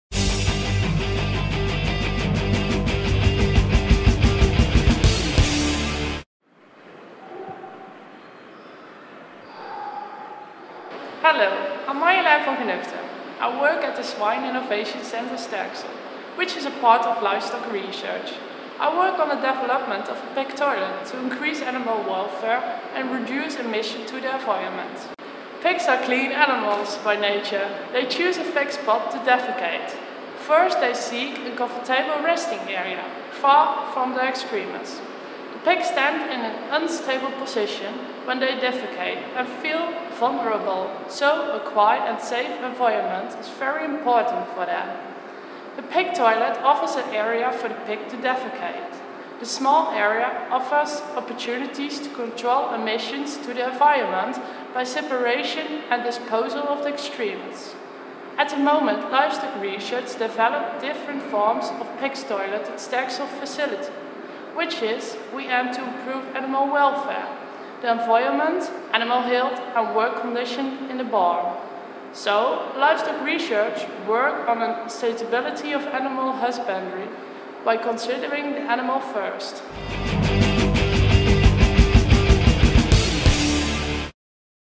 Research in one minute: the pig toilet
Wageningen Livestock Research, designed a pig toilet to improve pig welfare and reduce emissions of harmful substances into the environment.